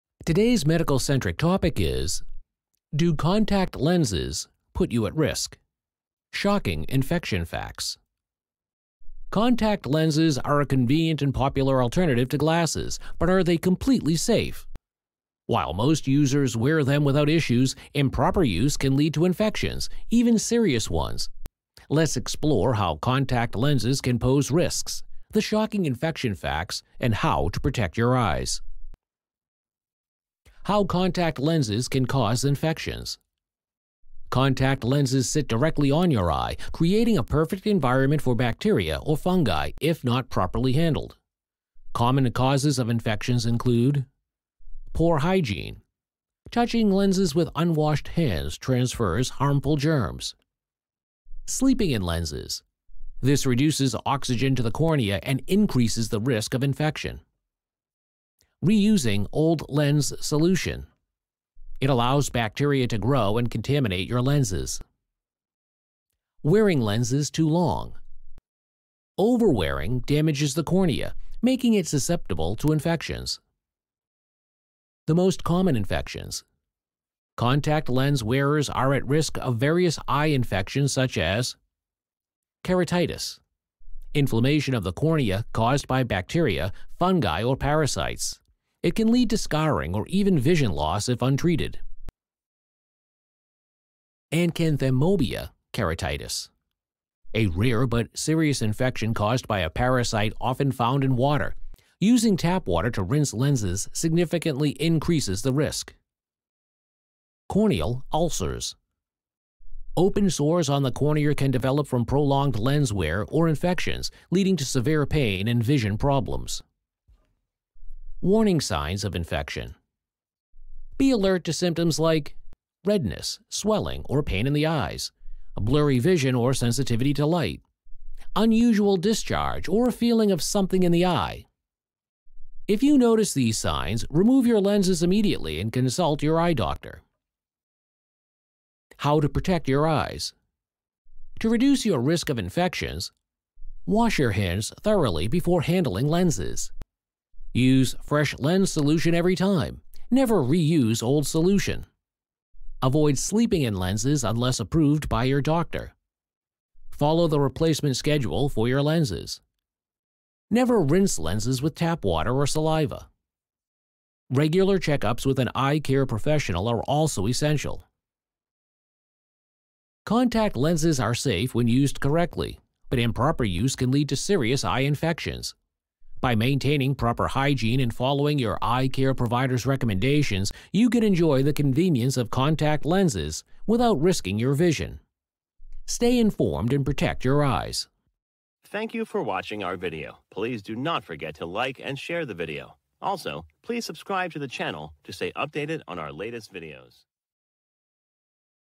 Eye Health Alert: Contact Lenses & Infections
**Do Contact Lenses Put You at Risk? Shocking Infection Facts**

Contact lenses are a popular alternative to glasses, offering convenience and better vision. However, improper use can lead to serious eye infections. Here's what you need to know to stay safe.

**How Do Infections Happen?**
Contact lenses sit directly on the cornea, creating a warm, moist environment ideal for bacteria and fungi. Common infections include bacterial keratitis and fungal infections, which can cause redness, pain, and even vision loss if untreated.

**Risk Factors to Watch**
Wearing lenses overnight, not cleaning them properly, or using tap water instead of lens solution increases infection risks. Disposable lenses are less risky when used as directed, but extending their use can invite trouble.

**How to Protect Your Eyes**
1. **Follow Proper Hygiene:** Always wash and dry your hands before handling lenses.
2. **Use Recommended Solutions:** Avoid tap water or homemade solutions, as they can introduce harmful microorganisms.
3. **Replace Lenses and Cases Regularly:** Overused cases and lenses are hotspots for bacteria.

**Symptoms to Act On**
If you experience redness, blurred vision, pain, or sensitivity to light, remove your lenses immediately and consult an eye care professional. Early intervention can prevent severe complications.

**Final Thought**
Contact lenses are safe when used responsibly. By practicing good hygiene and following your eye doctor's instructions, you can enjoy clear vision without compromising your eye health. Always prioritize eye safety—it’s worth the effort!